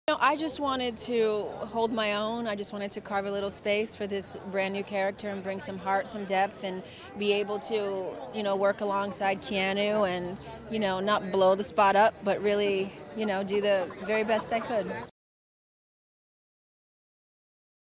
Halle Berry At 'John Wick 3' Special Screening
Halle Berry talks about her new role at the screening for 'John Wick: Chapter 3 Parabellum'.